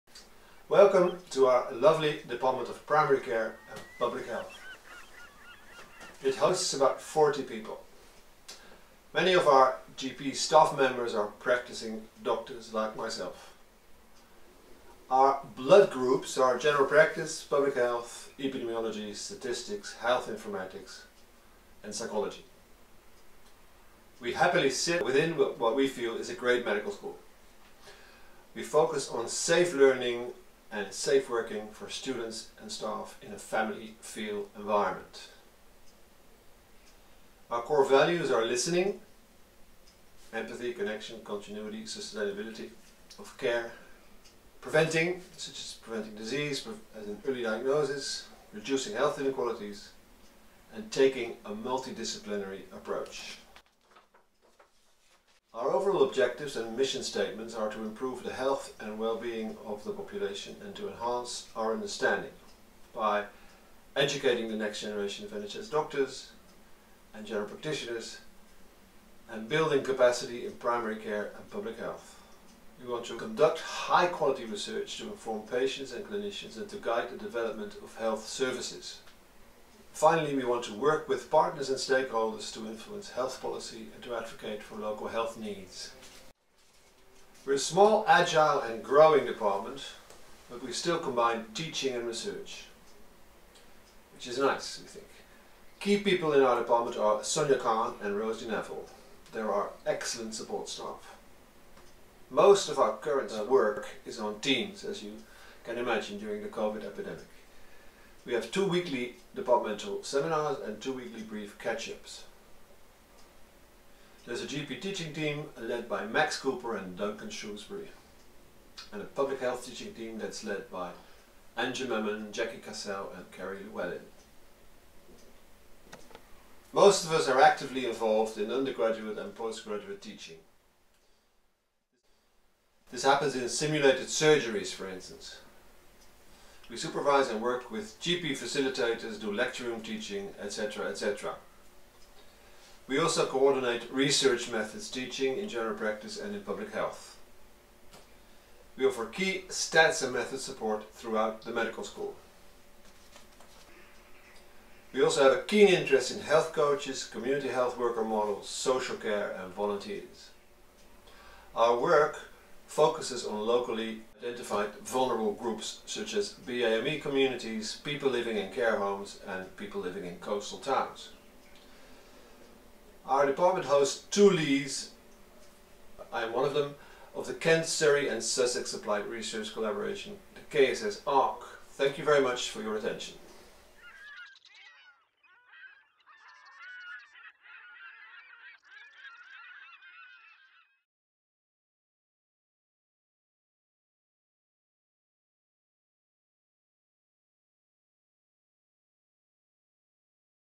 An introduction to Primary Care and Public Health at BSMS
Find out more about the Primary Care and Public Health department at BSMS in this introduction video. Prof Harm van Marwijk discusses the department's research areas and core values, and the role the department plays in teaching students at BSMS.